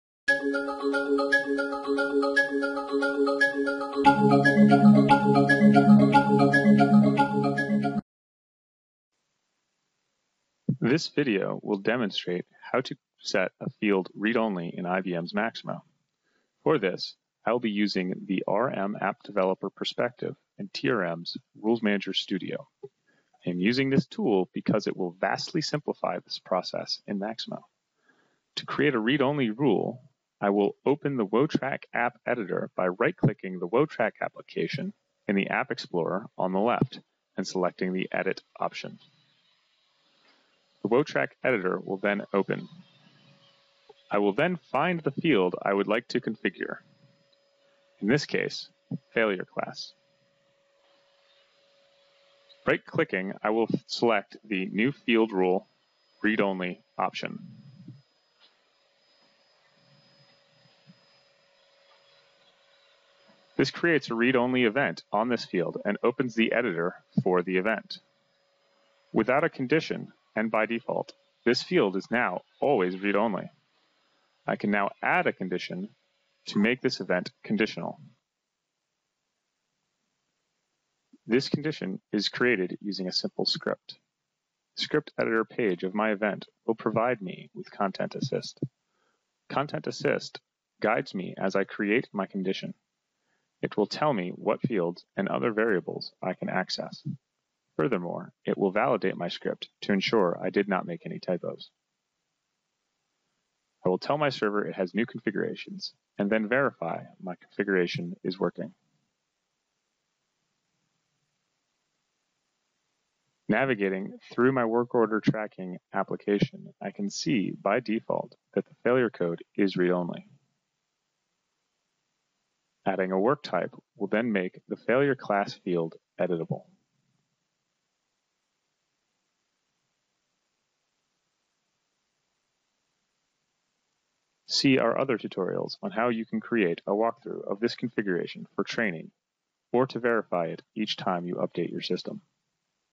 How to Set a Field ReadOnly in IBM Maximo | RulesManager | Total Resource Management
This video demonstrates how to set a field conditionally ReadOnly in IBM's Maximo Enterprise Asset Management.

For this, I use the TRM RulesManager App Developer Perspective in TRM's RulesManager Studio. I am using this tool because it will vastly simplify this process in Maximo.

To create a ReadOnly rule, I will open the WOTRACK App editor by right-clicking the WOTRACK application in the App Explorer on the left and selecting the Edit option.

The WOTRACK editor will then open. I will then find the field I would like to configure, in this case Failure Class, and right-click. I will then select the Field Event -_ ReadOnly option.

This creates a ReadOnly event on this field and opens the editor for the event.

Without a condition and by default, this field is now always ReadOnly. I can now add a condition to make this event conditional.

This condition is created using a simple script.

The script editor page of my event will provide me with content assist. Content assist guides me as I create my condition. It will tell me what fields, and other variables, I can access. Furthermore, it will validate my script to ensure I did not make any typos.

I will tell my server it has new configurations, and then verify my configuration is working.

Navigating through my Workorder Tracking application I can see by default that the failure code field is readonly.

Adding a worktype will then make the failure class field editable.

See our other tutorials on how you can create a walkthru of this configuration for training, or to verify it each time you update your system.